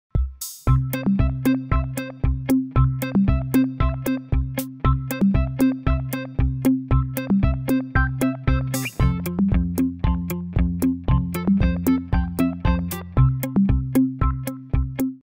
SPY X | Шпионский бинокль
Этот шпионский бинокль поможет увидеть предметы приближенными как днем, так и ночью!